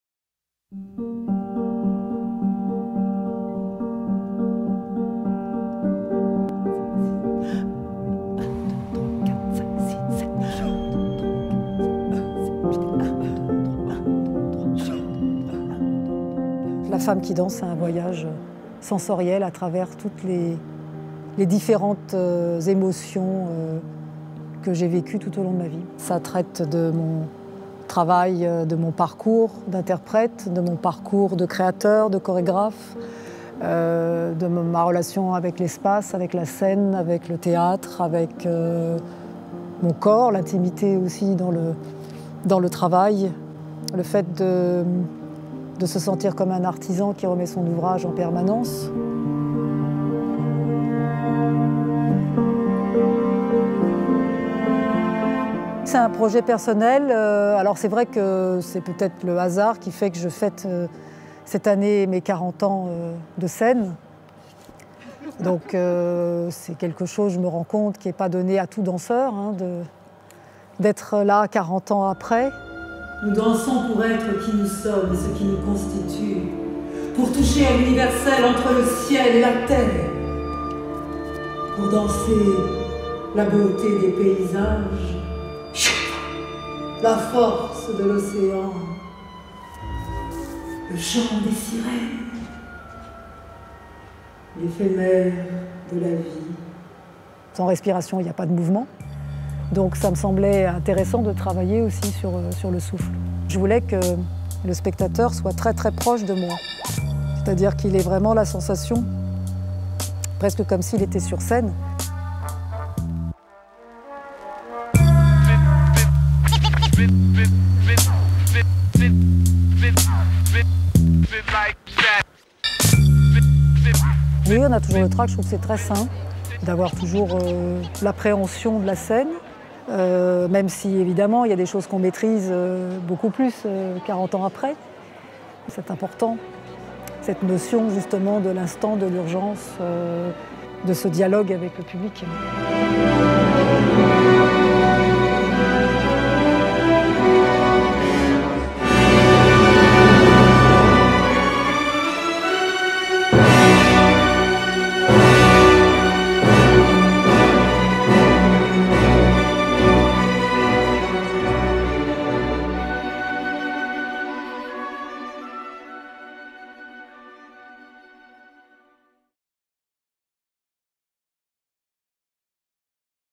Backstage #18 CCYF / PIETRAGALLA Une femme qui danse
Série BACKSTAGE réalisée pour le Centre Culturel Yves Furet de La Souterraine.
ccyf.fr
Marie-Claude PIETRAGALLA
La femme qui danse
Compagnie Théâtre du Corps